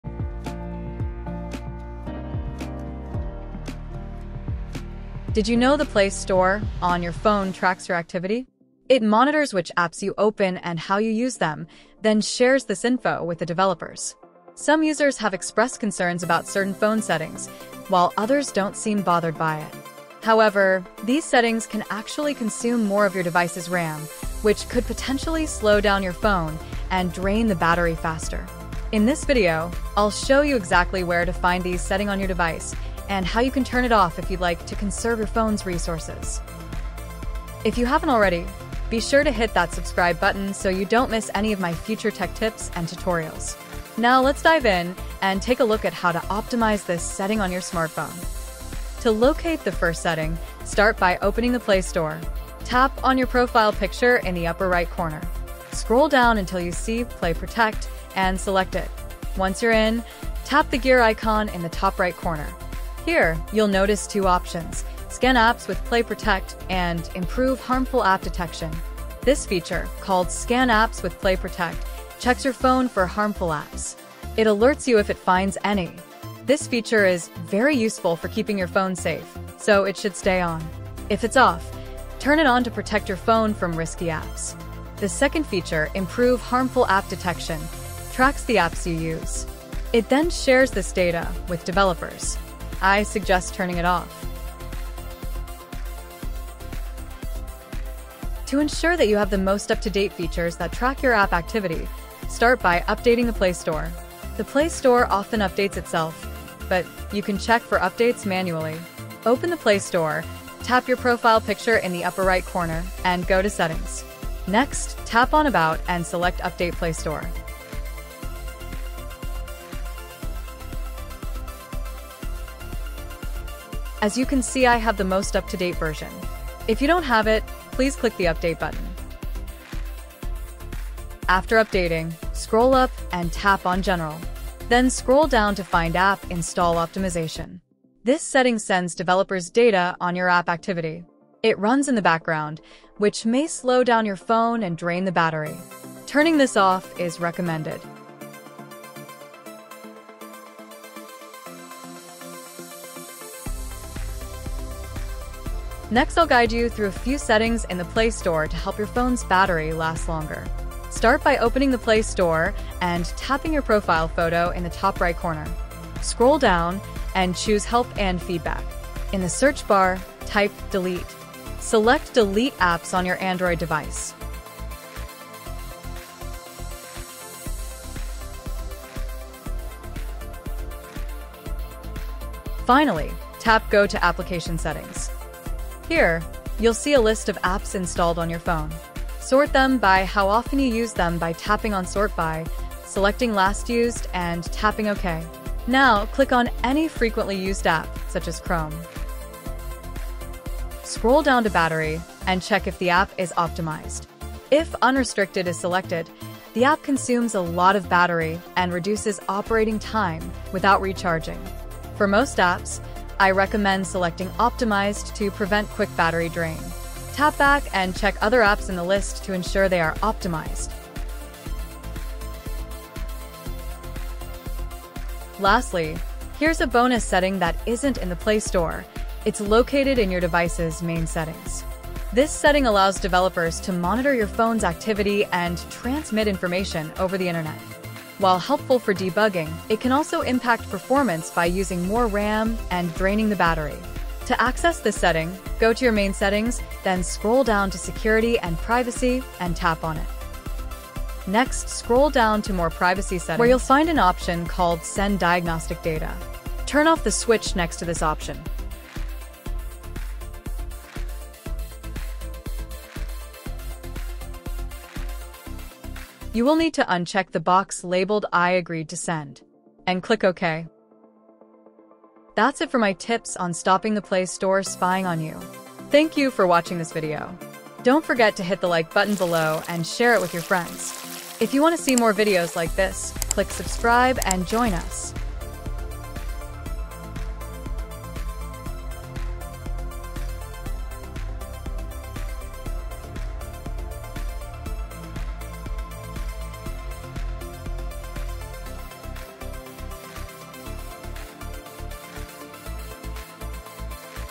How To Stop Play Store Spying on you
Take control of your privacy! Watch this video to learn which Play Store settings you need to change to keep your Android from spying on you.